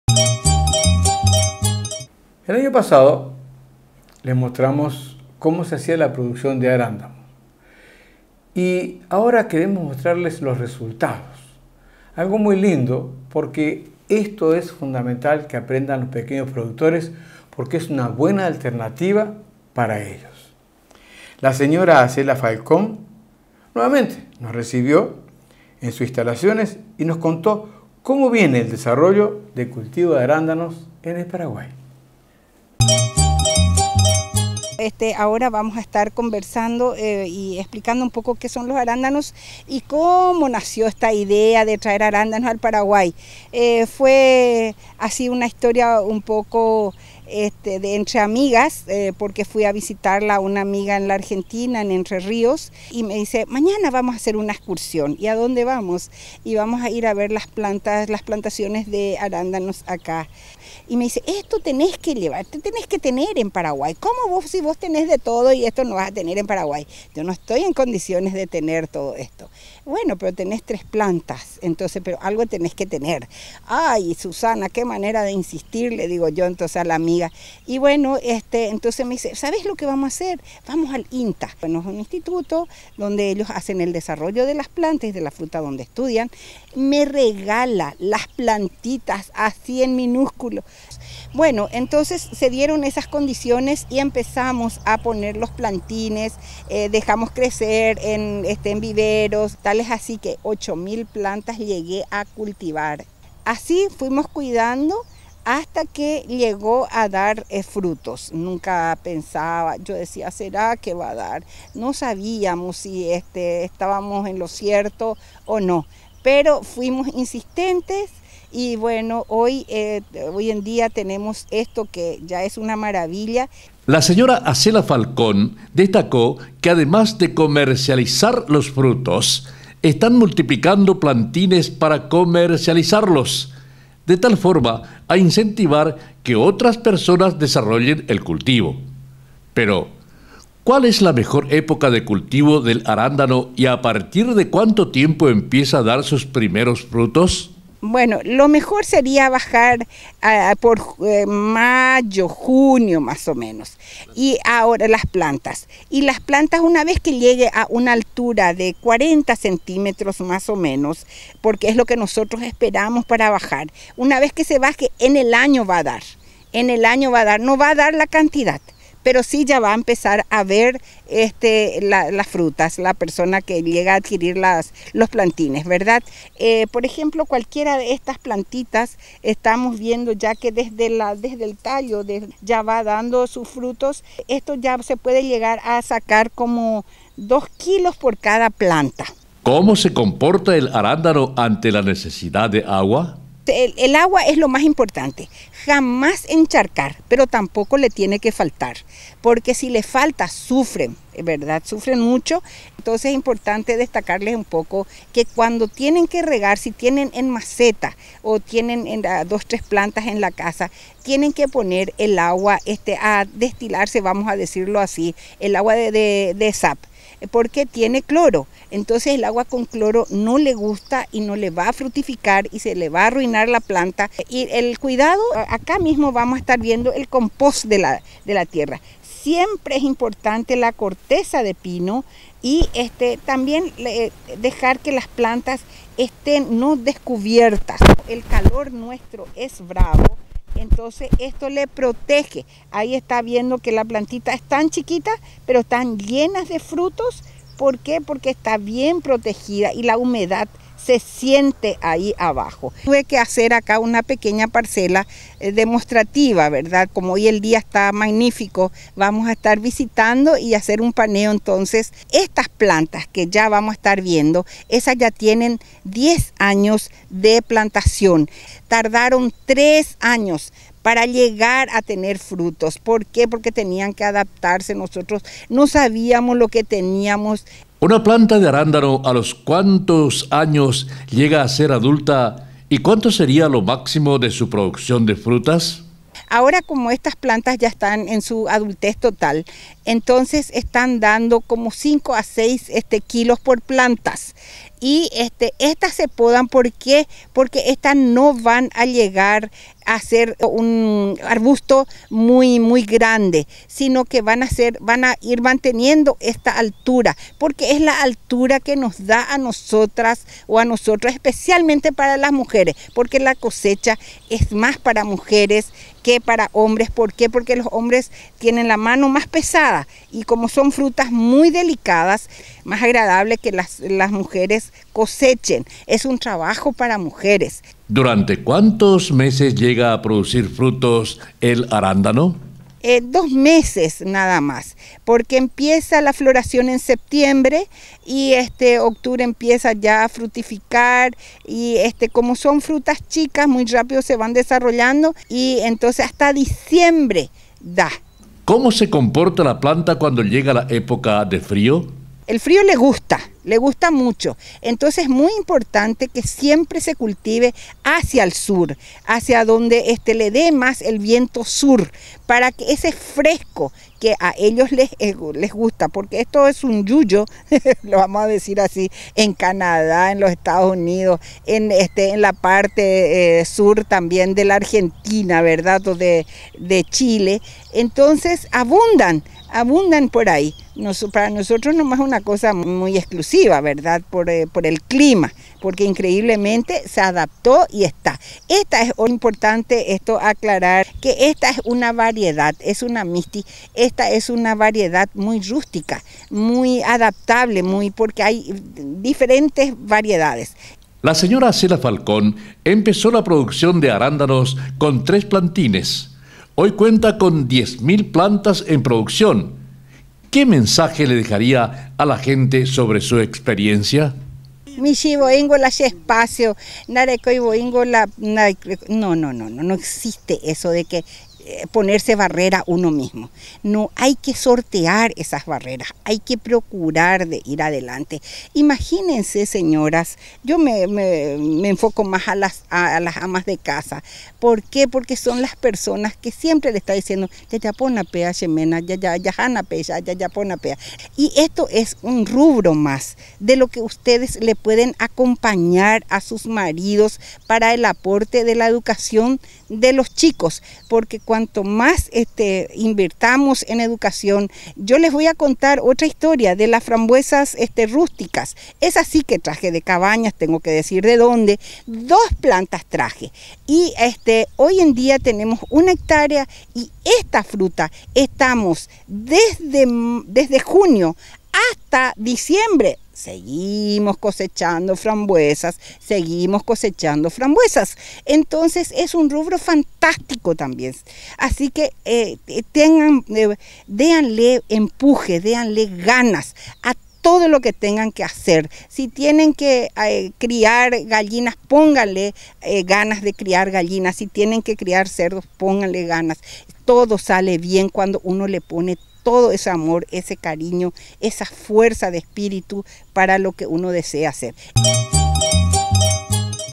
Situación y perspectivas del cultivo de arándano en el país
En  este material, la Sra. Acela Falcón describe las características de  este cultivo y los  potenciales beneficios  para el pequeño productor.